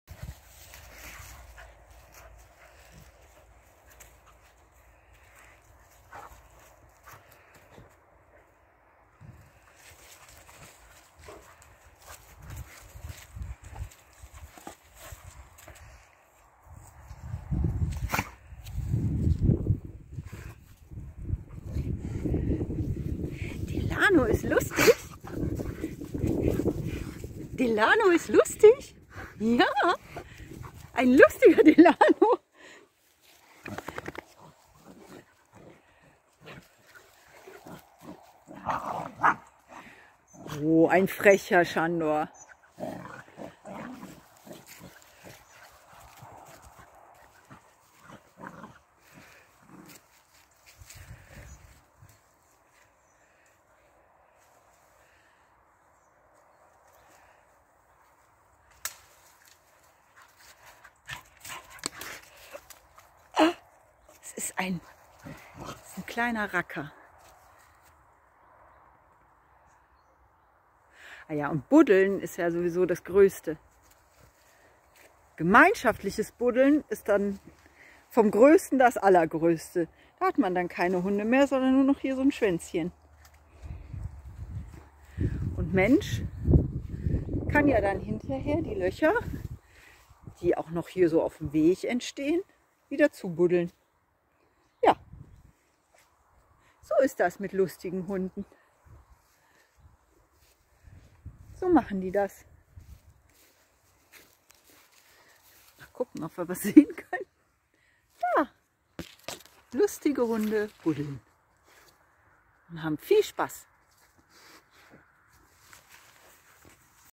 Übermütige und fröhliche Hunde
🥰 Klein Sandor rüpelt manchmal, aber Delano weiß immer gut mit ihm umzugehen. Und dann haben alle zusammen so viel Freude. 💚💙